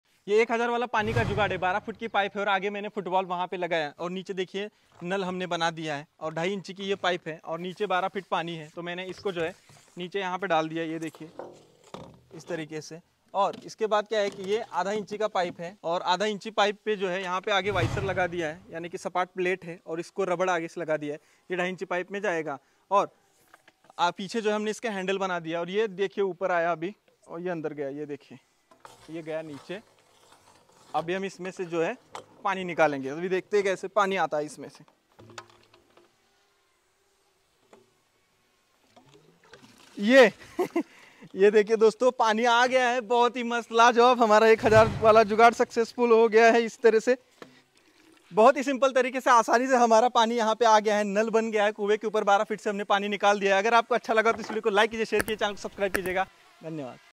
बिना लाइट के अब कुआँ से निकलेगा पानी💦Lallantop Jugaad | Indian Farmer
बिना लाइट के अब कुआँ से निकलेगा पानी,लल्लन टॉप जुगाड़

MY FAVOURITE EQUIPMENT:
🎤Second Mic
🧠Hard Drive
🧤Hand Gloves
💊First Aid Box